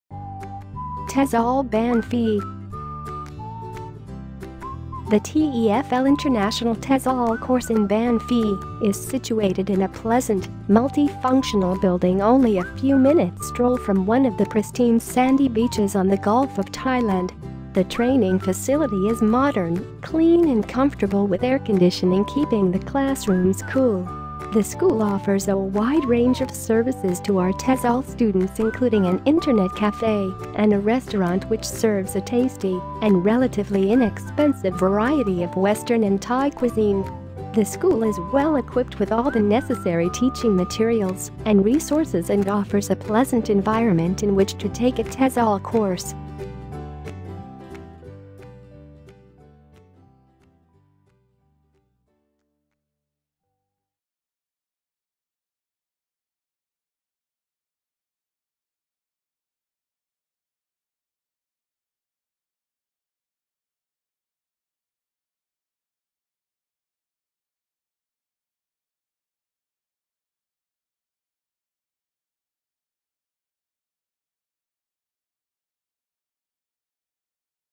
Welcome to Our TESOL School in Ban Phe, Thailand | Teach & Live abroad!
The TEFL International TESOL course in Ban Phe is situated in a pleasant, multi-functional building only a few minutes stroll from one of the pristine sandy beaches on the Gulf of Thailand.

The training facility is modern, clean and comfortable with air-conditioning keeping the classrooms cool. The school offers a wide range of services to our TESOL students including an internet café and a restaurant which serves a tasty and relatively inexpensive variety of Western and Thai cuisine.

The school is well-equipped with all the necessary teaching materials and resources and offers a pleasant environment in which to take a TESOL course.

Find out more about taking a TEFL course in Ban Phe